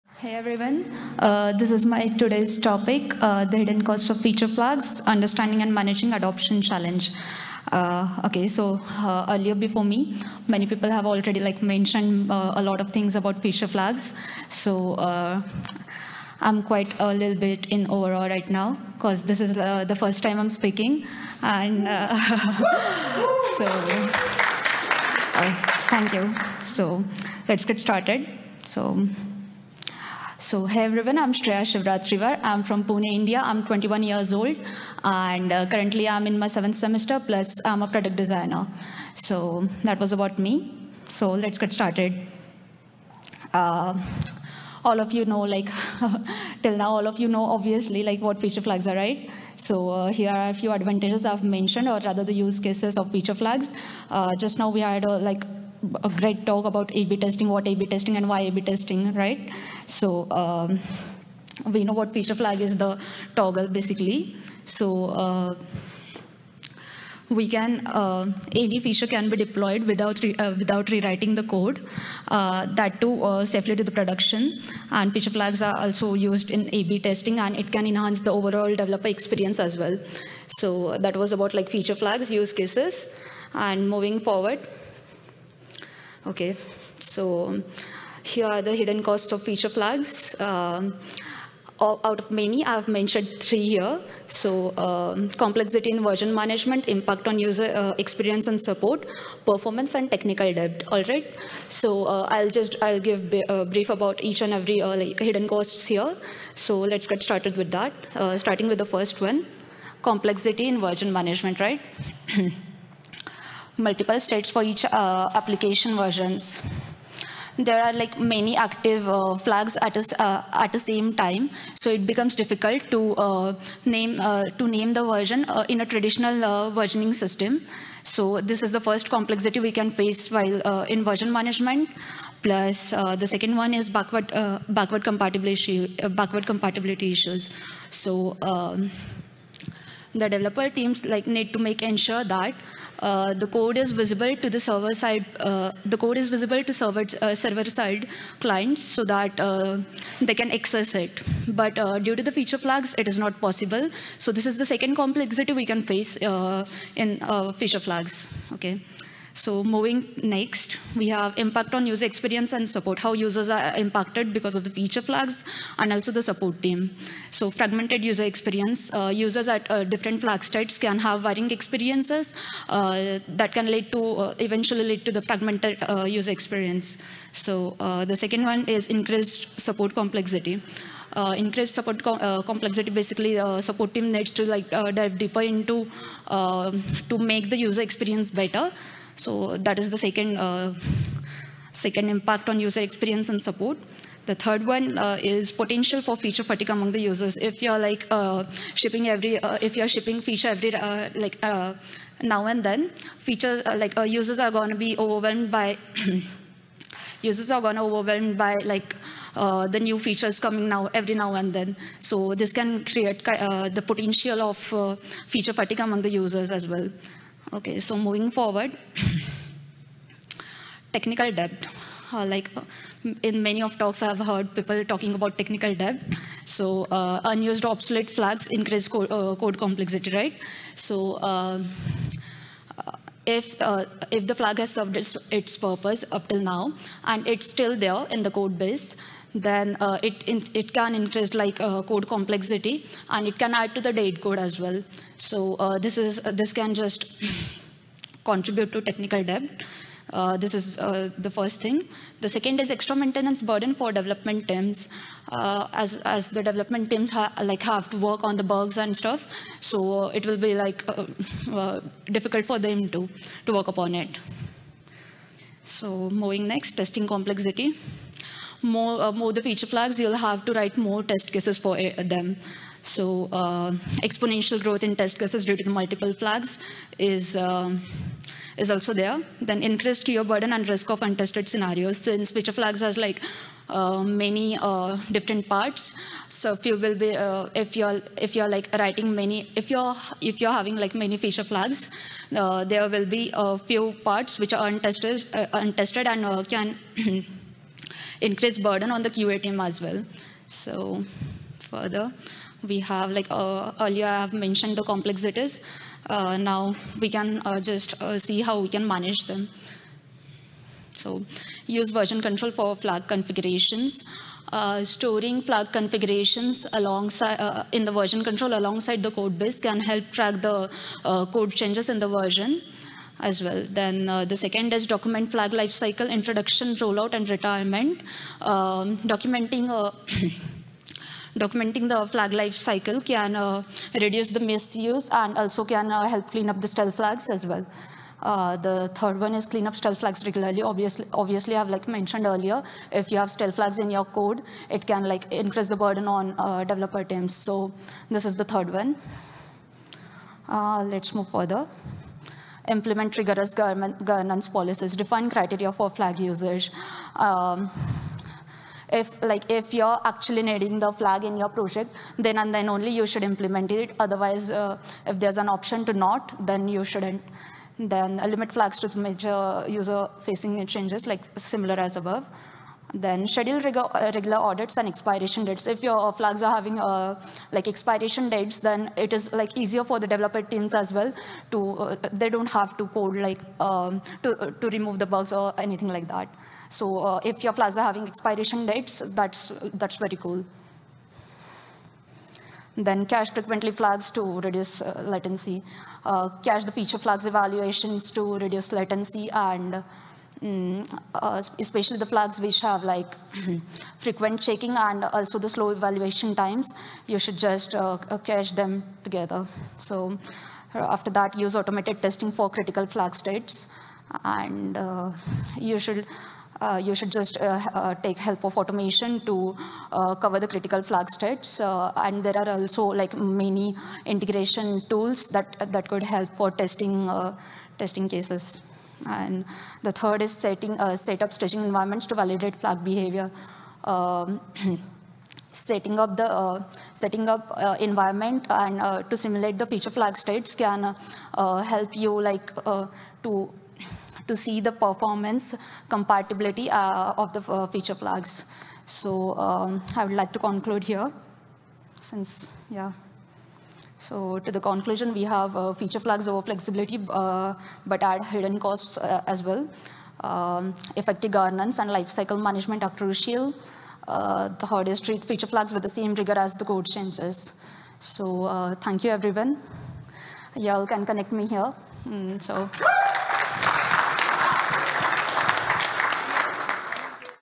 Lightning Talk: The Hidden Costs of Feature Flags: Understanding and Managin...- Shreya Shivratriwar
Lightning Talk: The Hidden Costs of Feature Flags: Understanding and Managing Adoption Challenges - Shreya Shivratriwar, Software Engineer

Feature flags offer powerful control over software releases, but they introduce hidden complexities in version management and user adoption that can catch development teams off guard. As application states multiply and the definition of a "version" blurs, teams struggle with maintaining compatibility and ensuring smooth updates across diverse platforms and user segments. To address these challenges, we propose a strategy of treating feature flag rollouts as code changes, favoring smaller, more frequent updates over large-scale alterations. This talk will unveil the often-overlooked costs of feature flags, explore their impact on versioning and user experience, and provide attendees with practical strategies for mitigating these issues. By the end, you'll have a clearer understanding of how to navigate the complex landscape of feature flag implementation, balancing the benefits of flexibility with the need for manageable, user-friendly software evolution.